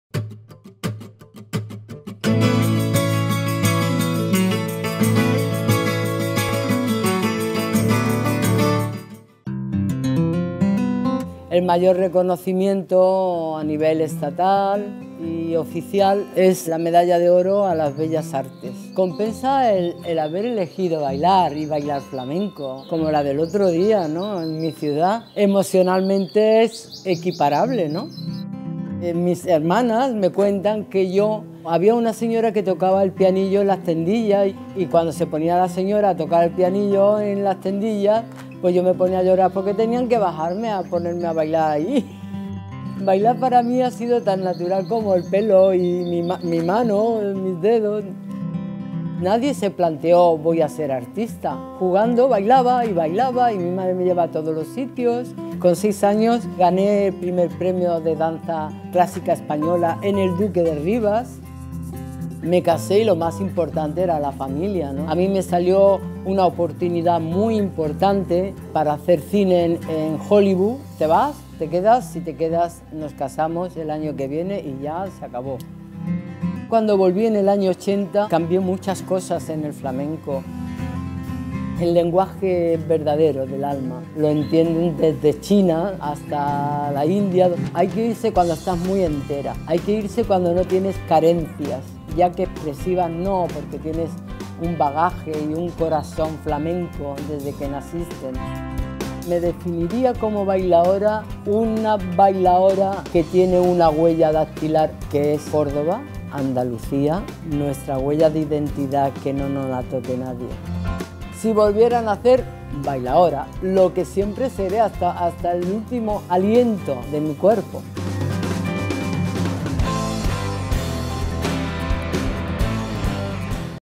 HDL Blanca del Rey, el flamenco como huella dactilar
La cordobesa Blanca del Rey fue una bailaora precoz. A 6 años empezó a demostrar que el don de artista lo lleva en la sangre y fue entonces cuando comenzó una trayectoria que le llevaría por todo el mundo. A los 14 debutó en un tablao de flamenco de Madrid y consiguió hacerse hueco en el mundo del flamenco. En el Corral de la Morería, uno de los templos sagrados del flamenco, fue donde se consagró, llegando a ser considerada por los entendidos como la mejor bailaora del momento en 1984. A lo largo de su trayectoria Blanca del Rey ha recibido multitud de distinciones, siendo para ella una de las más importantes la Medalla de Oro a las Bellas Artes. Su Córdoba natal siempre ha sido la inspiración de su arte. Hoy en día está retirada del baile, pero no desvinculada del flamenco, ya que ejerce como coreógrafa en la actualidad.